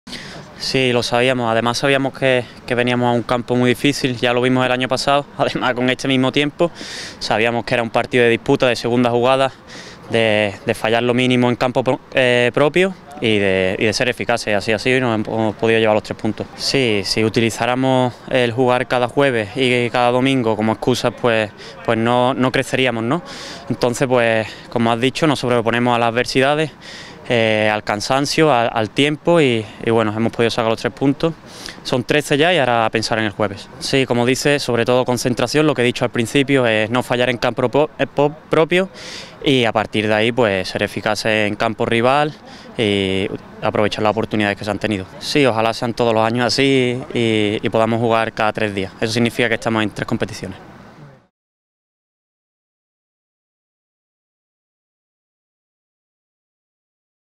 Getafe FC vs Granada CF | Valoraciones de Carlos Neva postpartido
Valoraciones postpartido Getafe FC vs Granada CF del encuentro de la jornada 7 de LaLiga Santander